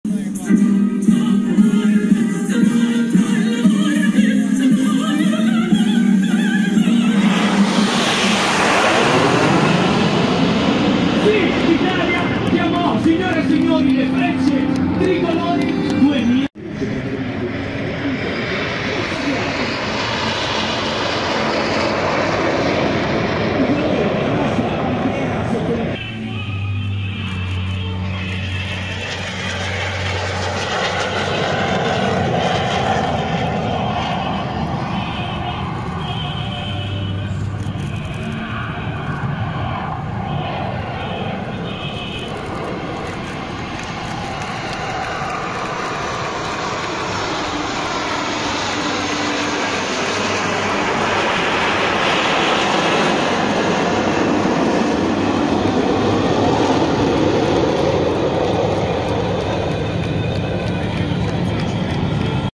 Il sorvolo, la mischia e l'abbraccio. Le tre figure spettacolari delle Frecce Tricolori a Jesolo
La Pattuglia acrobatica nazionale ha lasciato a bocca aperta mezzo milione di appassionati di volo e curiosi con le sue figura mozzafiato allo Jesolo Air Show. (Video Claudio Vianello